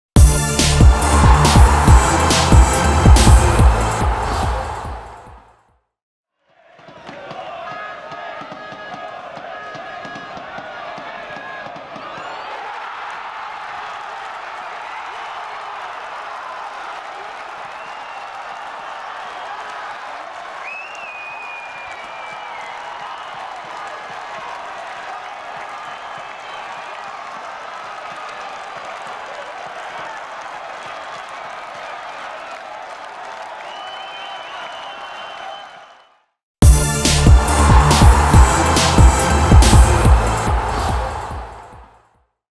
Knallertor von Deran Toksöz (FC Eintracht Norderstedt) | ELBKICK.TV
Knallertor zur Rettung

Wenn alles verloren scheint, dann braucht es Fußballer wie Deran Töksez. Der Spieler in Diensten des FC Eintracht Norderstedt rettet sein Team mit diesem Wahnsinnstreffer aus 20 Metern in die Verlängerung des Pokalfinals gegen Altona 93. In der 89. Minute!

Wenn auch Ihr kürzlich ein fantastisches Tor gefilmt habt, das man gesehen haben muss, meldet Euch bei uns per Mail an oder schreibt uns bei Facebook.